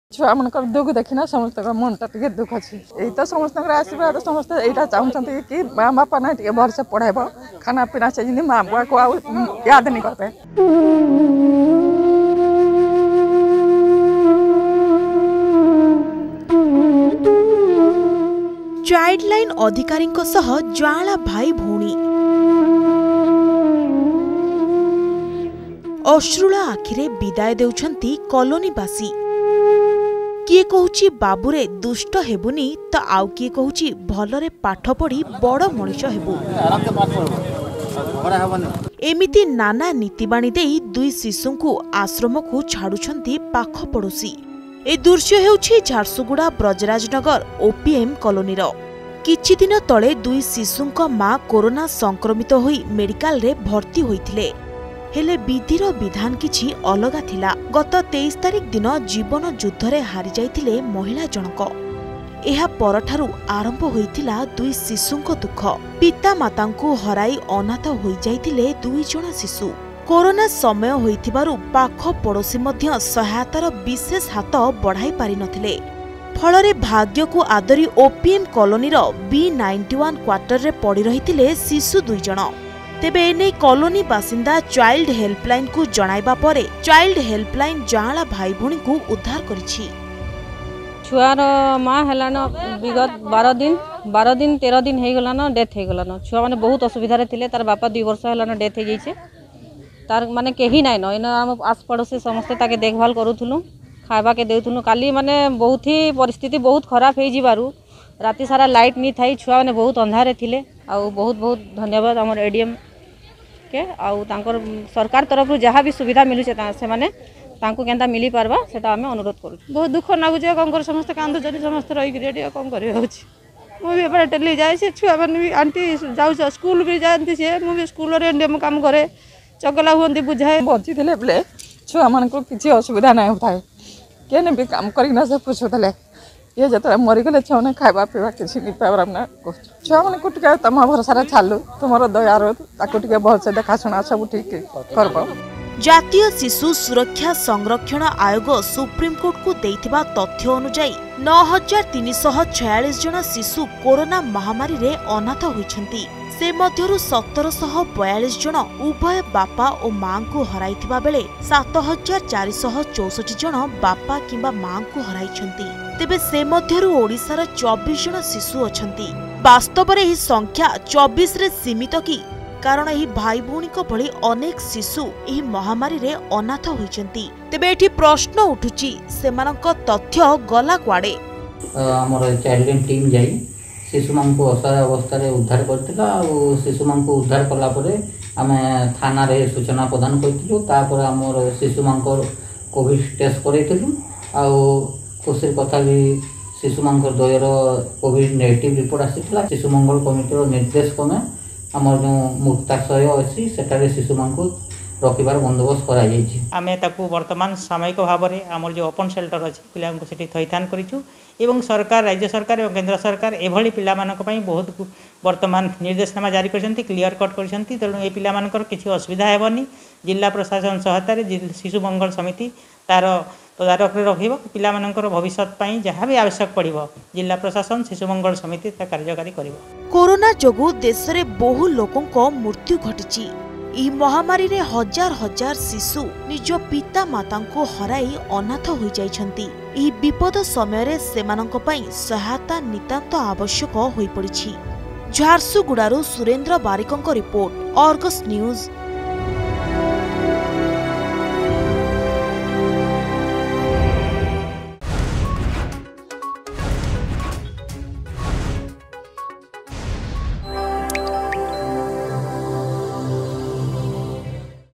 Child Helpline Rescues Orphan Twins Whose Parents Had Died Of Covid-19 In Jharsuguda
ଚାଇଲଡ଼ ଲାଇନ ଅଧିକାରୀଙ୍କ ସହ ଯାଆଁଳା ଭାଇ ଭଉଣୀ । ଅଶୃଳ ଆଖିରେ ବିଦାୟ ଦେଉଛନ୍ତି କଲୋନିବାସୀ । କିଏ କହୁଛି ବାବୁରେ ଦୁଷ୍ଟ ହେବୁନି ତ କିଏ କହୁଛି ଭଲରେ ପାଠ ପଢି ବଡ ମଣିଷ ହେବୁ